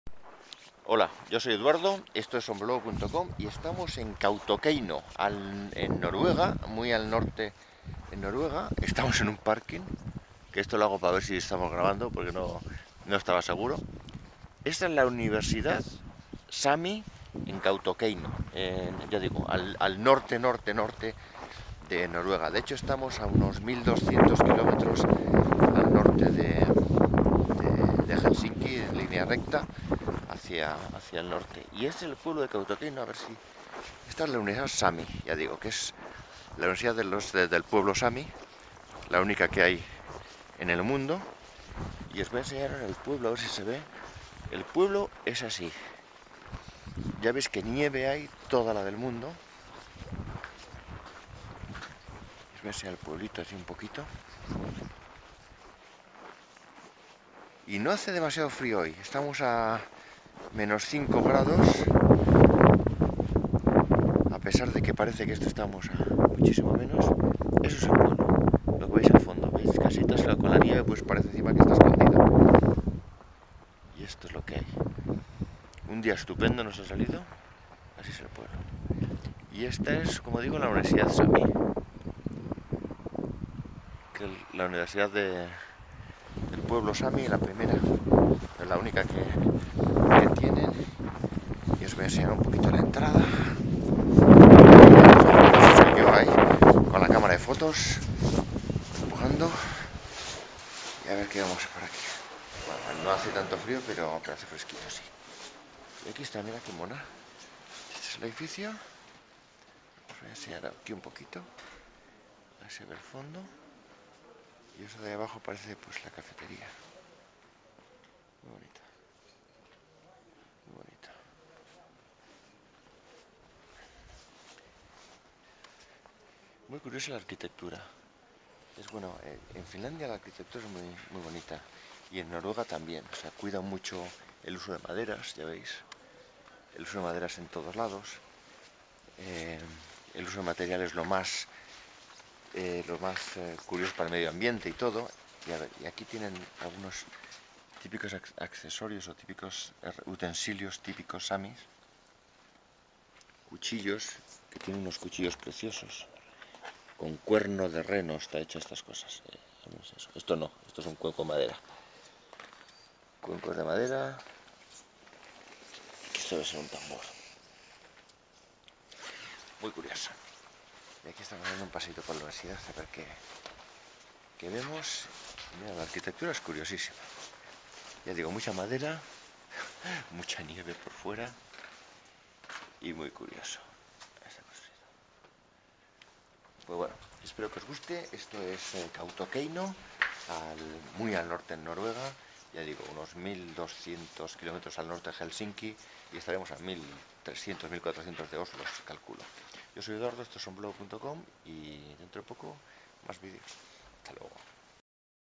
Universidad Sami en Noruega
- En la ciudad de Kautokeino, en Noruega, visitando la Universidad Sami.